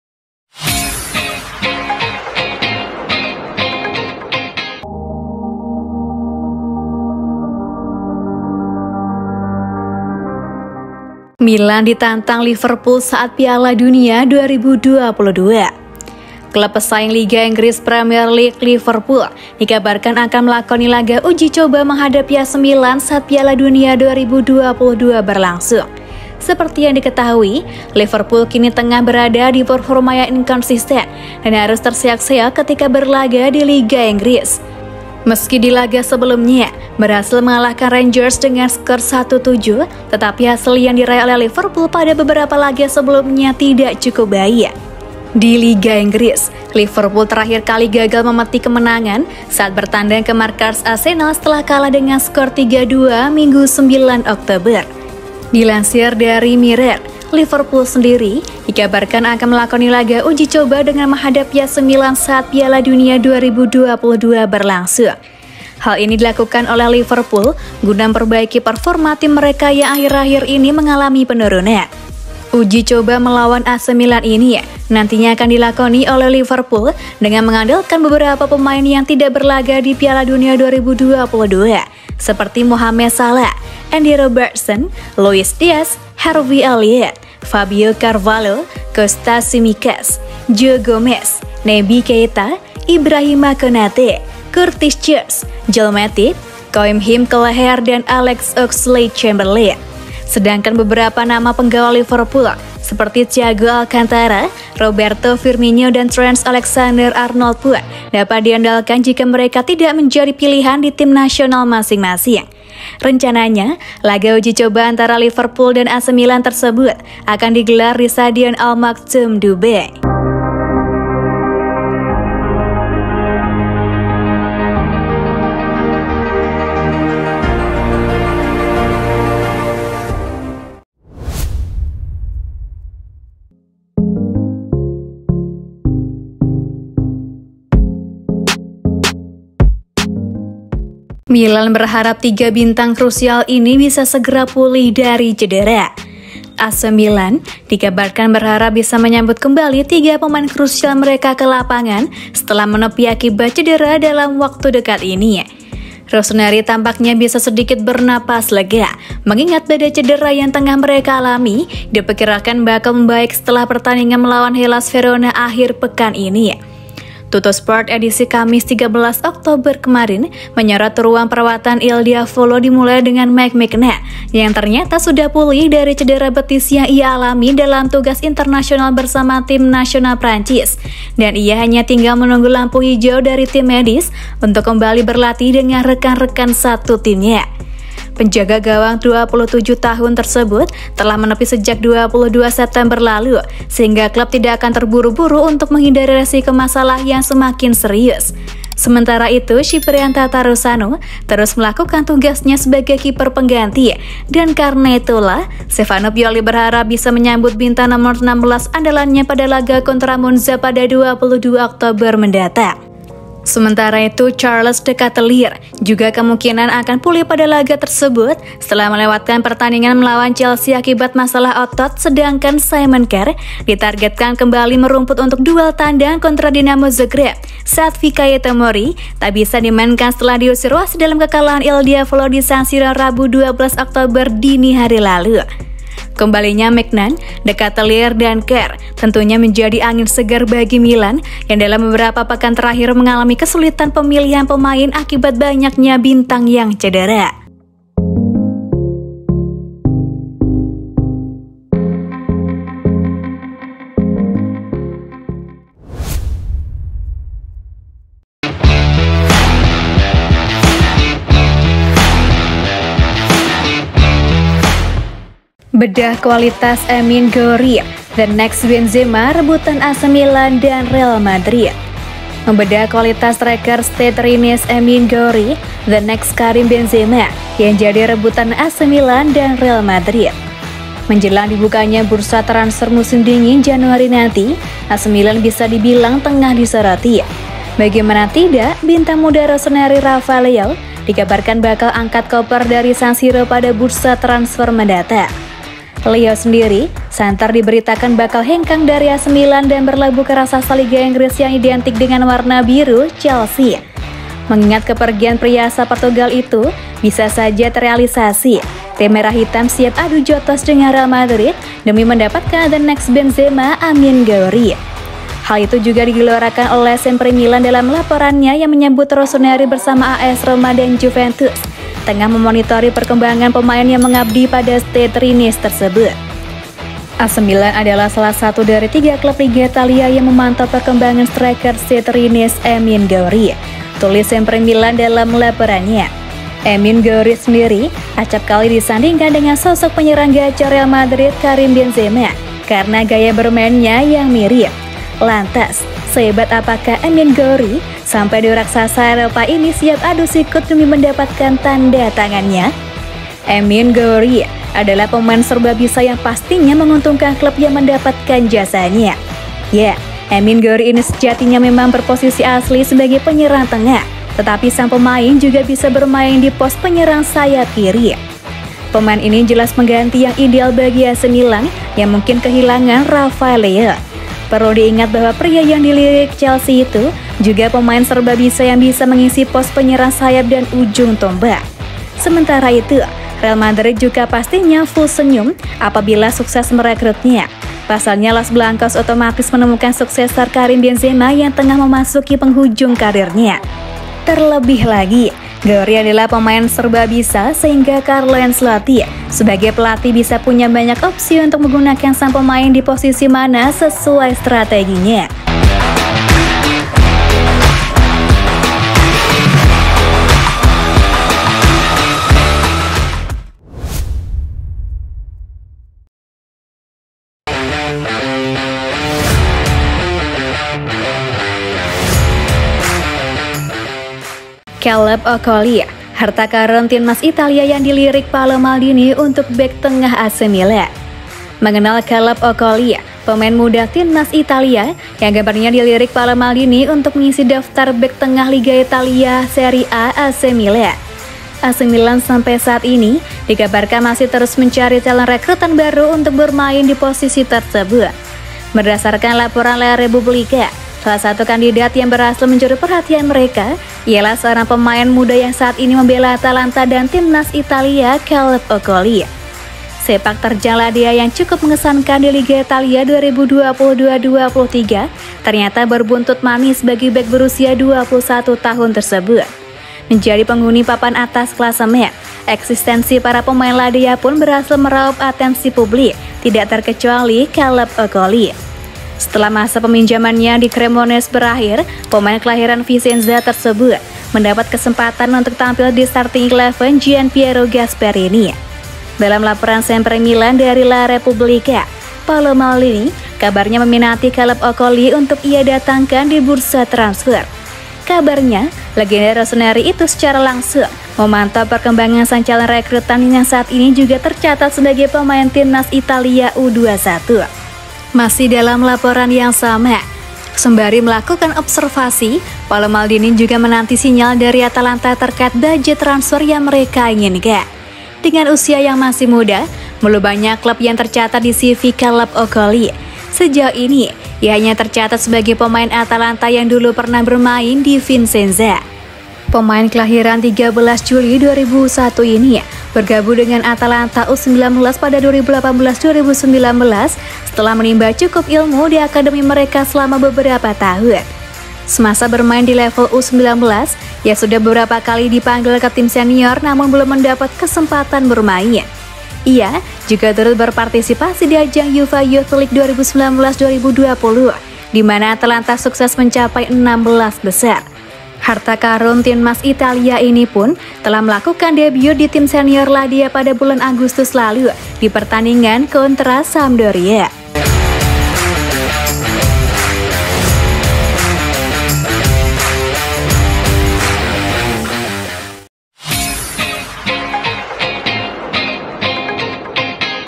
MILAN DITANTANG LIVERPOOL UNTUK NAIKKAN KUALITAS❗️AMINE GOUIRI, THE NEXT BENZEMA MILAN📌FORZA MILAN
1. Milan diantang Liverpool saat Piala Dunia 2022
2. Milan Berharap Tiga Bintang Krusial Ini Bisa Segera Pulih Dari Cedera
3. Bedah Kualitas Amine Gouiri, 'The Next Benzema' Rebutan AC Milan dan Real Madrid
4. Caleb Okoli, Harta Karun Timnas Italia yang Dilirik Paolo Maldini untuk Bek Tengah AC Milan